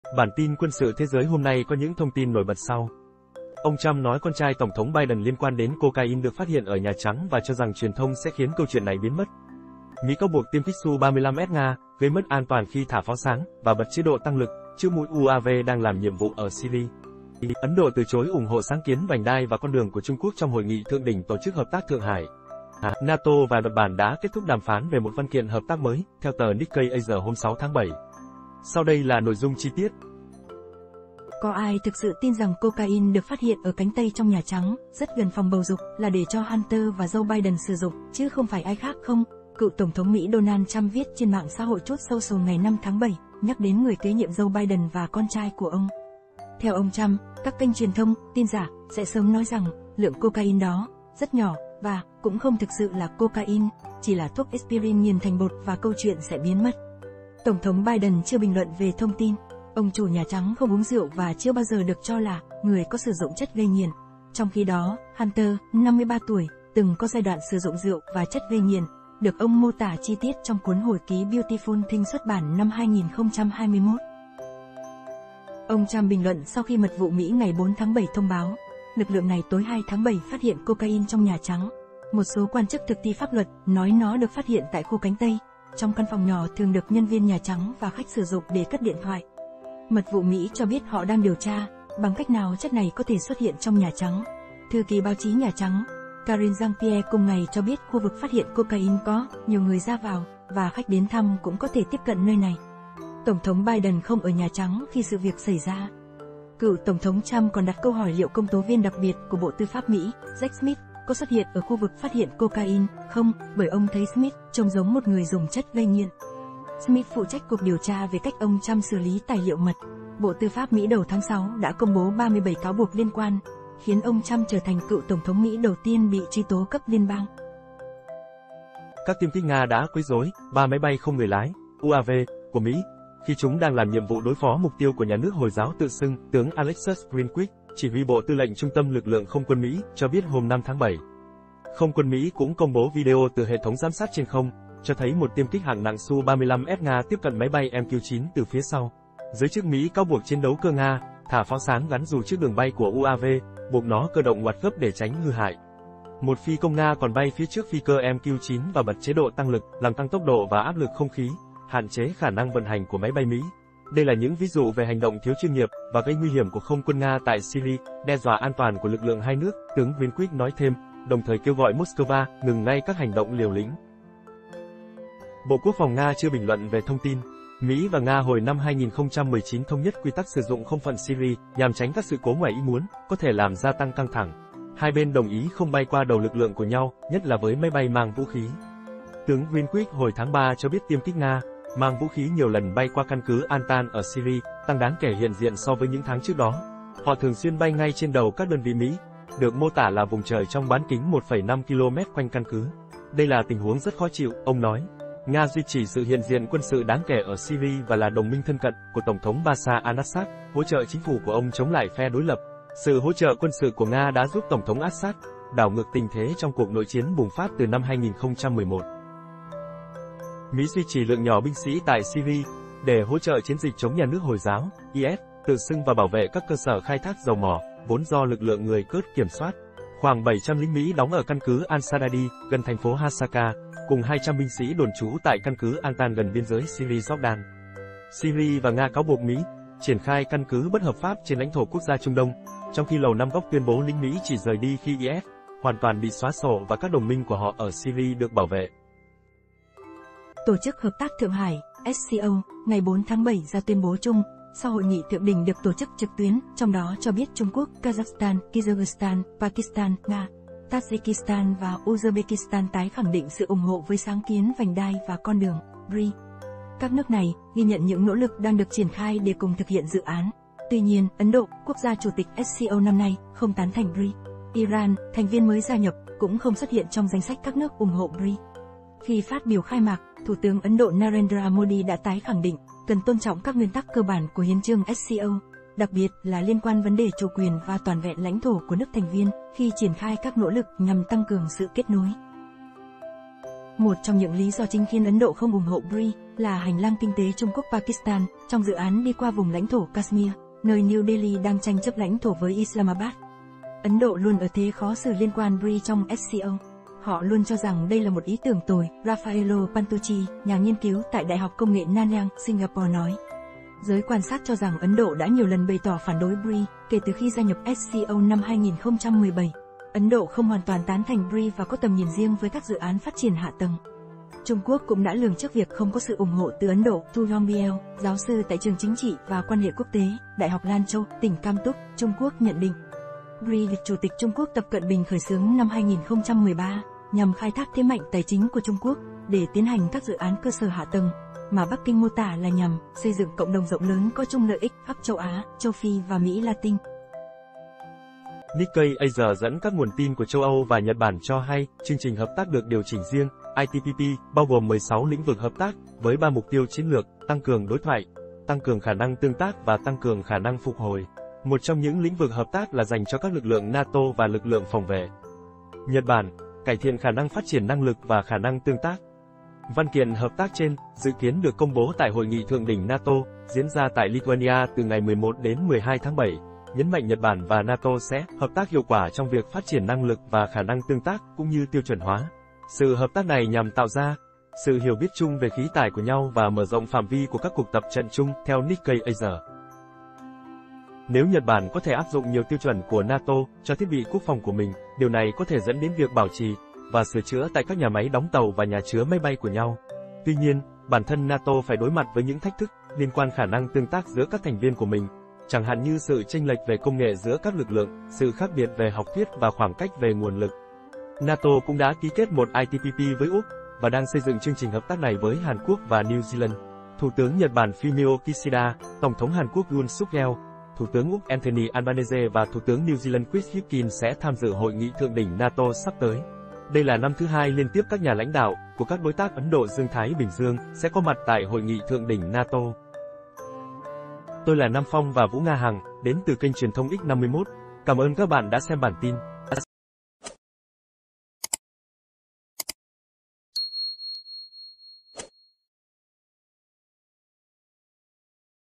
ÔNG TRUMP CÔNG KÍCH VỤ COCAINE Ở NHÀ TRẮNG | MỸ CÁO BUỘC NGA QUẤY RỐI UAV MỸ TẠI SYRIA | ẤN ĐỘ KHÔNG
"Có ai thực sự tin rằng cocaine được phát hiện ở Cánh Tây trong Nhà Trắng, rất gần Phòng Bầu dục, là để cho Hunter và Joe Biden sử dụng, chứ không phải ai khác không", cựu tổng thống Mỹ Donald Trump viết trên mạng xã hội Truth Social ngày 5 tháng 7, nhắc đến người kế nhiệm Joe Biden và con trai của ông.